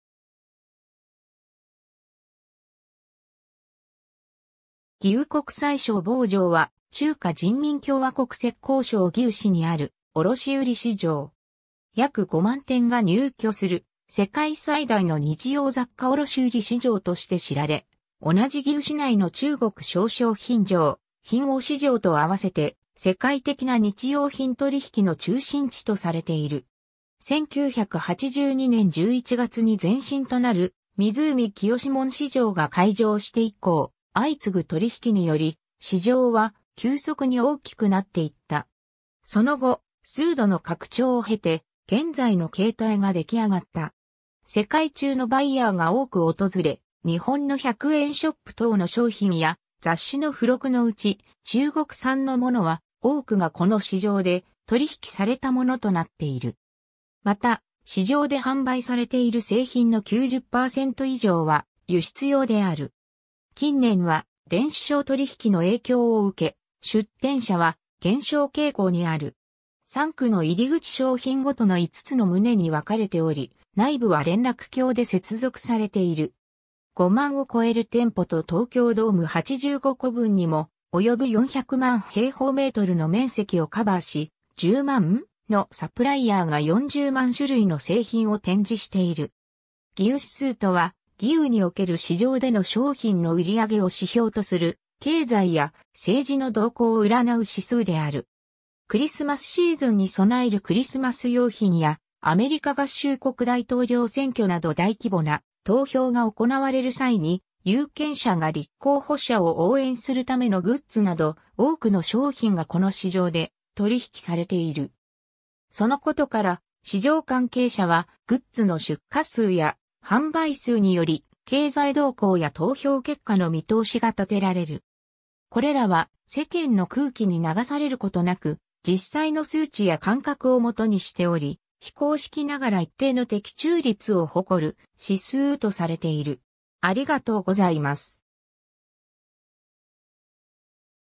義烏国際商貿城
義烏国際商貿城（イーウーこくさいしょうぼうじょう、簡体字中国語: 义乌国际商贸城、通称：福田市場）は、中華人民共和国浙江省義烏市にある卸売市場。
約5万店が入居する世界最大の日用雑貨卸売市場として知られ、同じ義烏市内の中国小商品城、賓王市場と合わせて、世界的な日用品取引の中心地とされている。
 1982年11月に前身となる湖清門市場（湖清门市场）が開場して以降、相次ぐ取引により、市場は急速に大きくなっていった。
その後、数度の拡張を経て、現在の形態ができあがった。
 世界中のバイヤーが多く訪れ、日本の100円ショップ等の商品や雑誌の付録のうち、中国産のものは多くがこの市場で取引きされたものとなっている。
また、市場で販売されている製品の90%以上は輸出用である。
 近年は電子商取引の影響を受け、出店者は減少傾向にある。
 三区の入口 商品ごとの5つの棟に分かれており、内部は連絡橋で接続されている。
5万を超える店舗と東京ドーム85個分にも及ぶ400万平方メートルの面積をカバーし、10万のサプライヤーが40万種類の製品を展示している。
 義烏指数（イーウーしすう、Yiwu Index）とは、義烏における市場での商品の売り上げを指標とする、経済や政治の動向を占う指数である。
クリスマスシーズンに備えるクリスマス用品や、アメリカ合衆国大統領選挙など大規模な投票が行われる際に有権者が立候補者を応援するためのグッズなど、多くの商品がこの市場で取引きされている 。
そのことから、市場関係者は、グッズの出荷数や販売数により経済動向や投票結果の見通しが立てられる。
これらは世間の空気に流されることなく、実際の数値や感覚を元にしており、非公式ながら一定の的中率を誇る指数とされている。